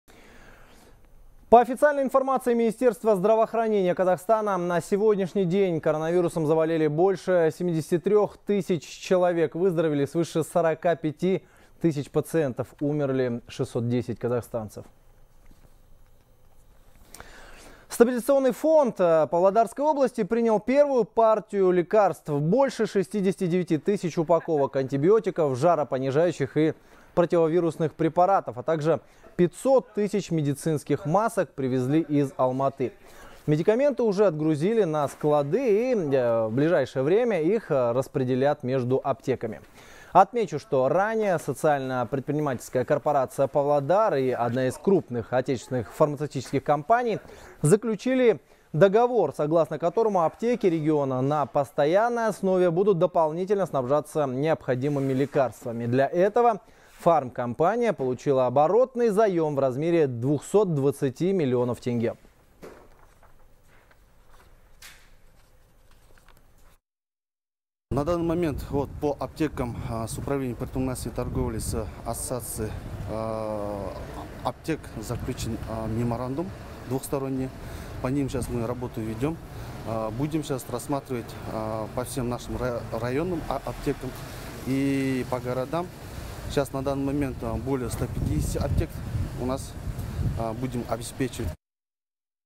Стабфонд Павлодарской области получил партию лекарств
Стабилизационный фонд Павлодарской области принял первую партию лекарств, сообщает корреспондент «Хабар 24».